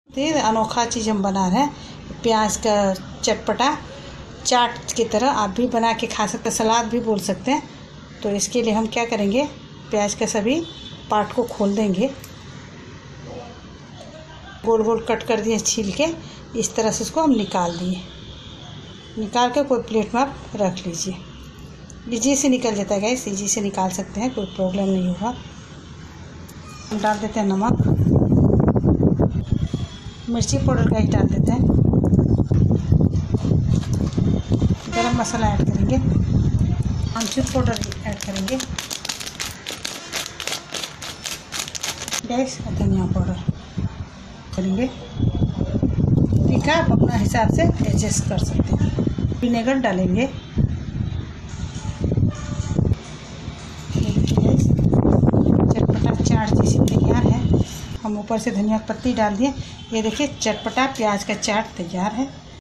Dhaba Style Laccha Pyaz Recipe | Masala Pyaz | Chatpata Onion Salad |Lacha Onion Salad
ingredients:
onion
red chilli powder
salt
lemon juice
garam masala
aamchur
green chilly
vinegar
chaat masala
coriander leaves